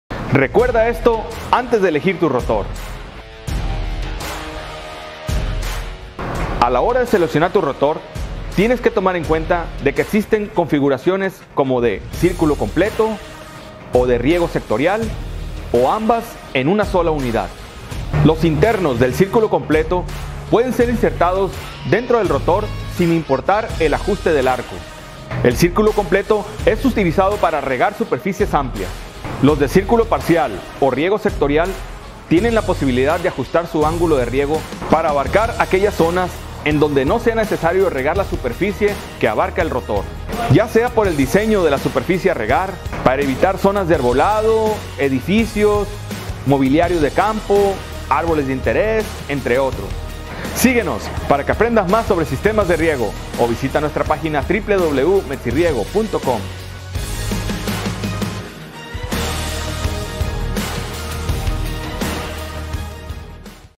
RECUERDA ESTO ANTES DE ELEGIR UN ROTOR
A la hora de seleccionar un rotor hay que tener también en cuenta que puede ser de círculo completo, de ángulo de riego sectorial, o modelos con ambas configuraciones disponibles en la misma unidad.

Síguenos para aprender más sobre sistemas de riego o visita nuestro blog en mexiriego.com